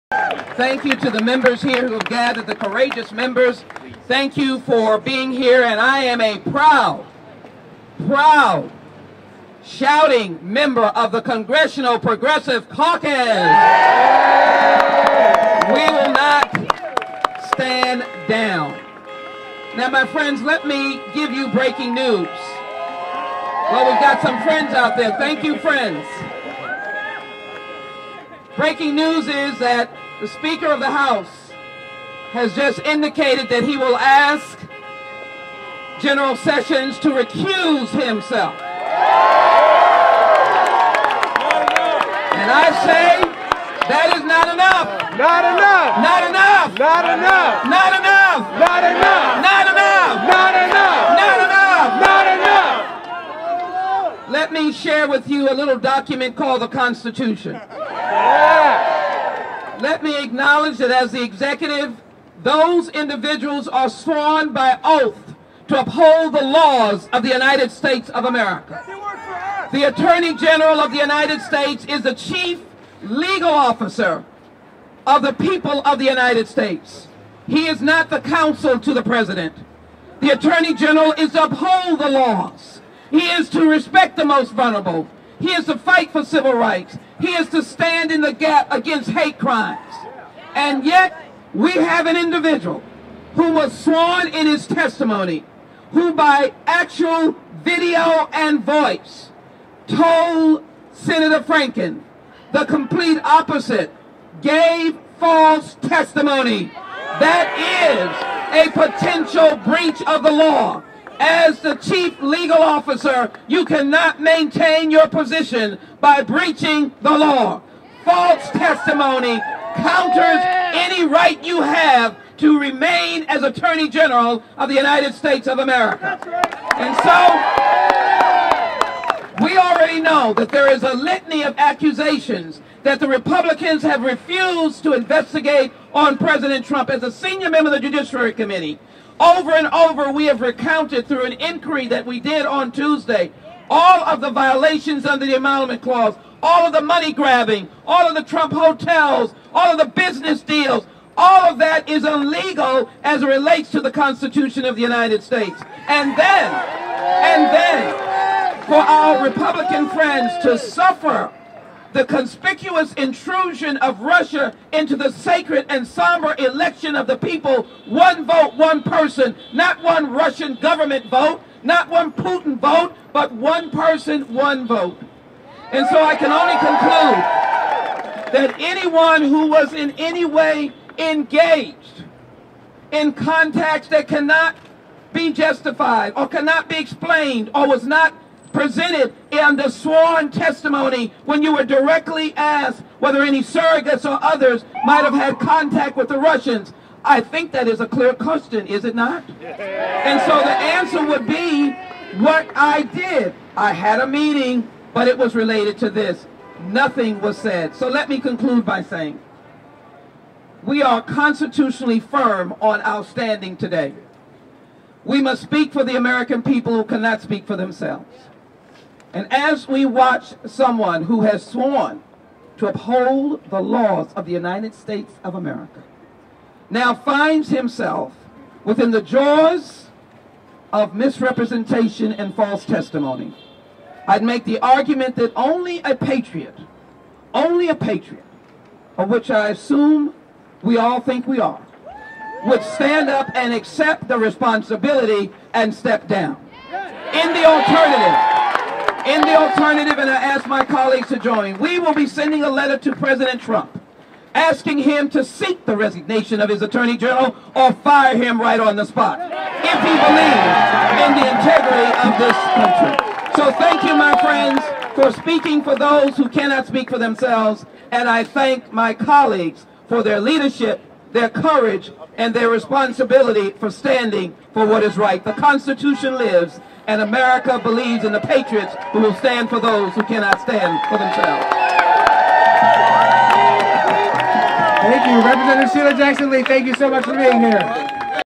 Sheila Jackson Lee Speaks Out Against Sessions
Congresswoman Sheila Jackson Lee spoke out against Attorney General Jeff Sessions Thursday to a crowd of hundreds of activists outside the DOJ.

Protesters gathered chanting "Lock him up!" and "fire him if he doesn't resign!" to express their frustration and mistrust of recently confirmed Attorney General Jeff Sessions. The activists say Sessions' contact with Russian ambassadors that he failed to disclose during his confirmation hearing demonstrate a cozy and clandestine relationship between the Trump administration and Russia.

Photos and additional footage may be available upon request.